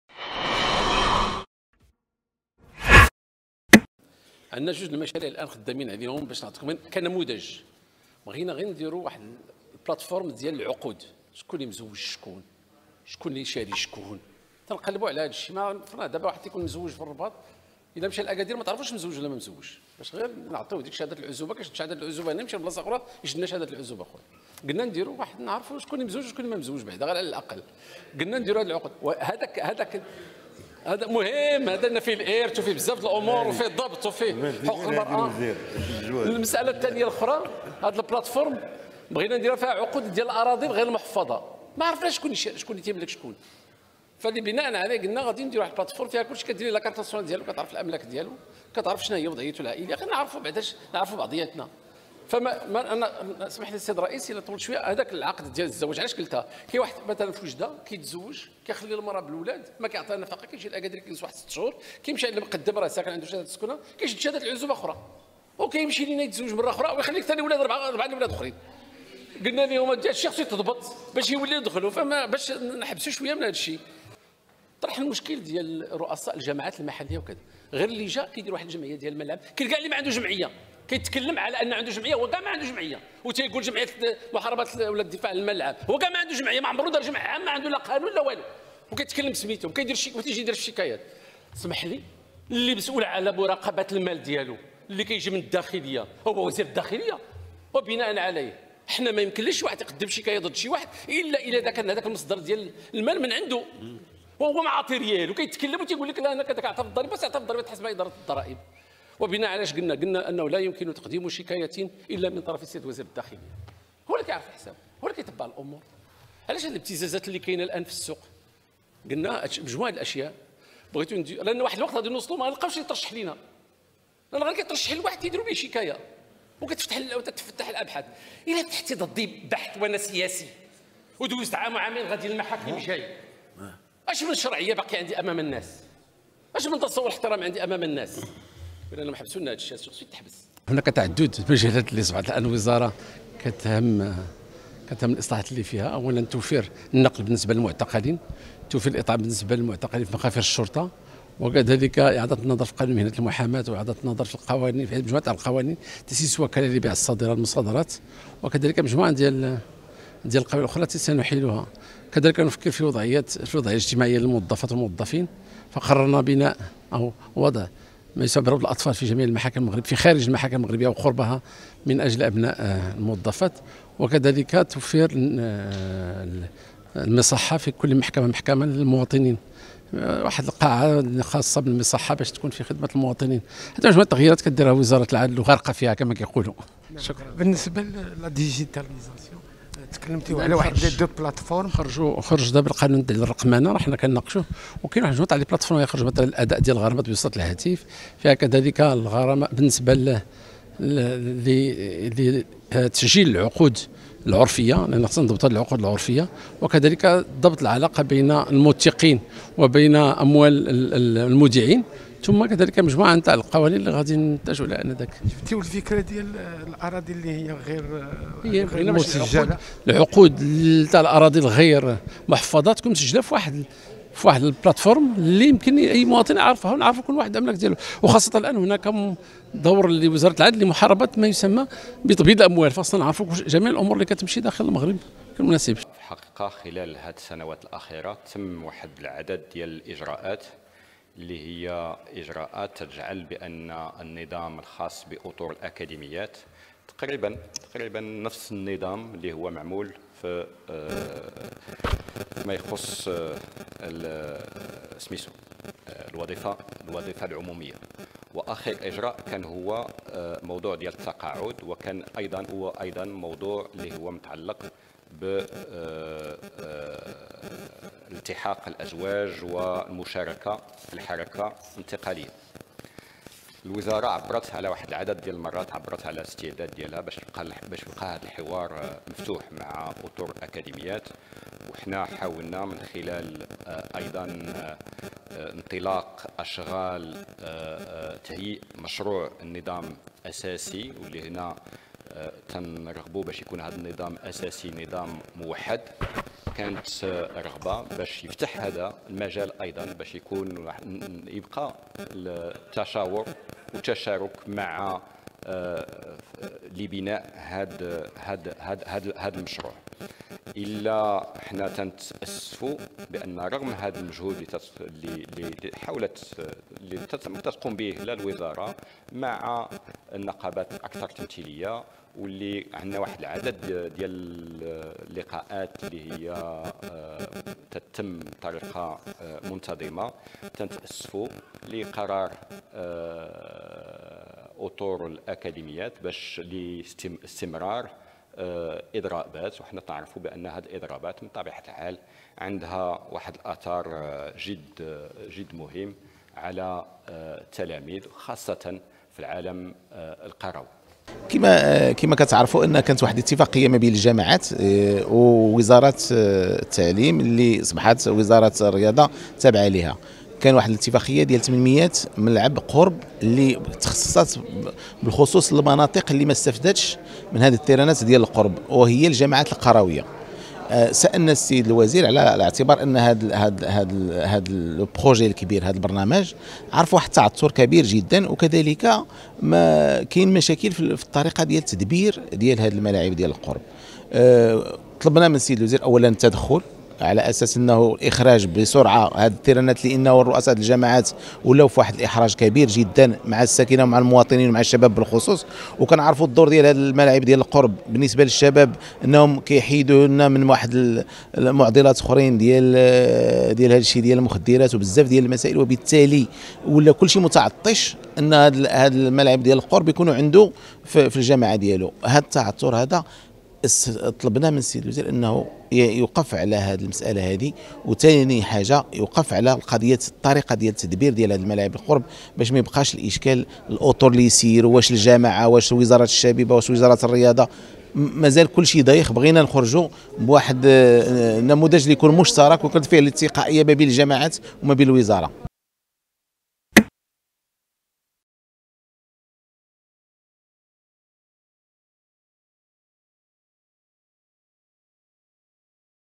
وهبي يعلن عن مشروع منصة للعقود لتفادي الاحتيال على شواهد العزوبية‎‎
Le ministre de la Justice, Abdellatif Ouahbi a annoncé la mise en point d’un projet de plate-forme numérique dédié aux époux mariés afin d’éviter la polygamie illégale et la délivrance des faux certificats de célibat.